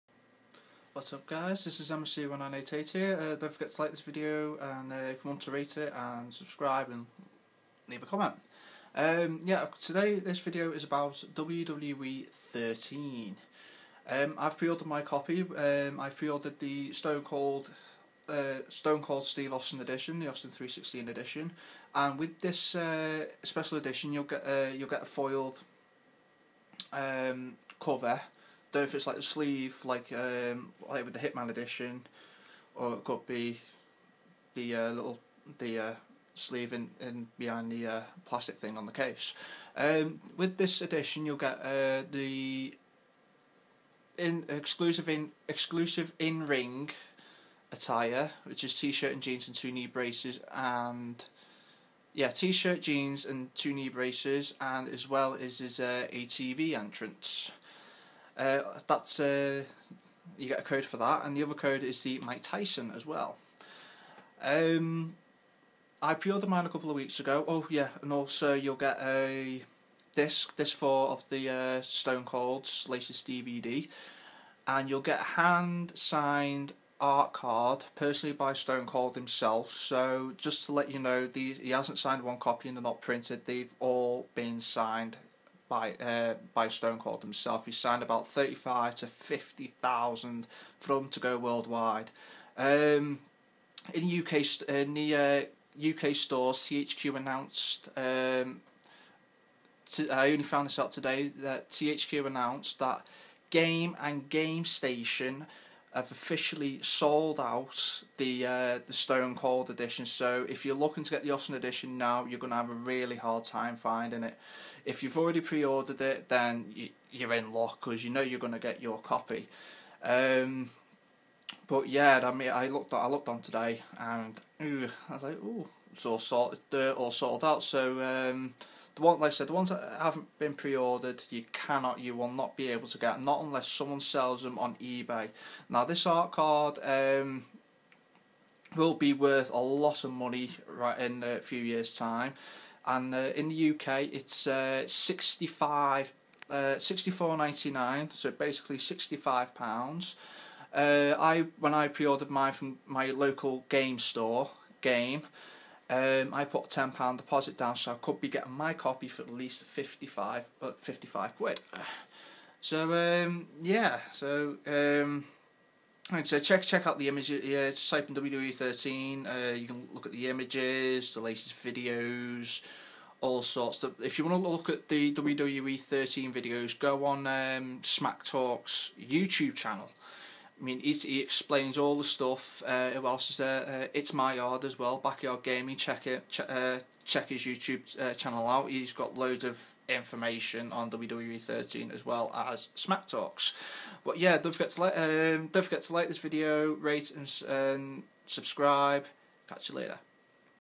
WWE 13 Austin 3:16 collectors edition
WWE 13 Austin 3:16 collectors limited edition game and gamestaion's preorder of the austin edition has been sold out in the uk for more wwe 13 check out Smacktalks and backyardgaming here's the links